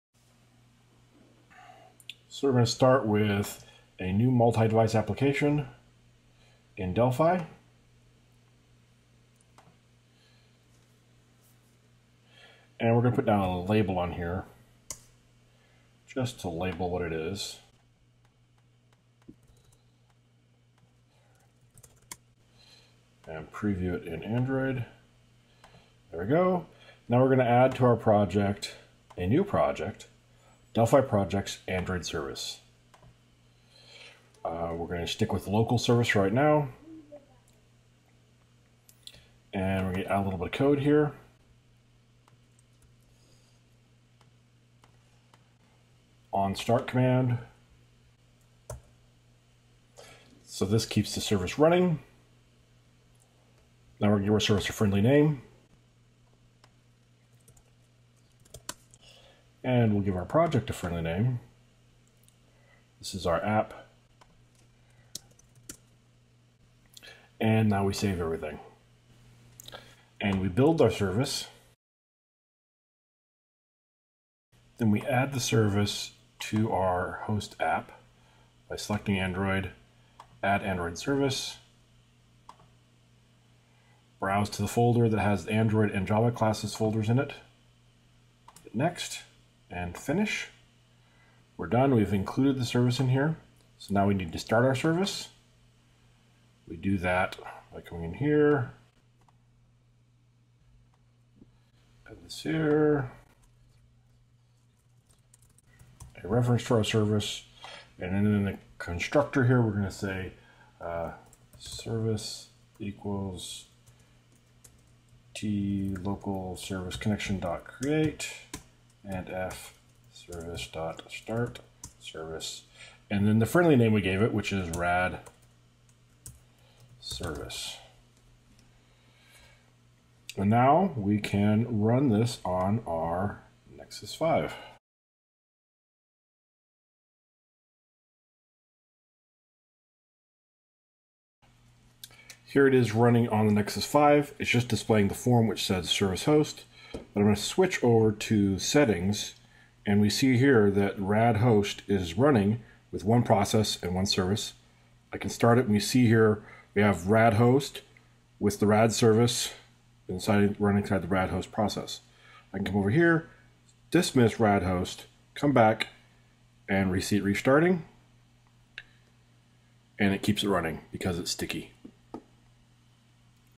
Delphi 10 Seattle Android Services Intro
Very brief introduction to building Android Services with Delphi 10 Seattle.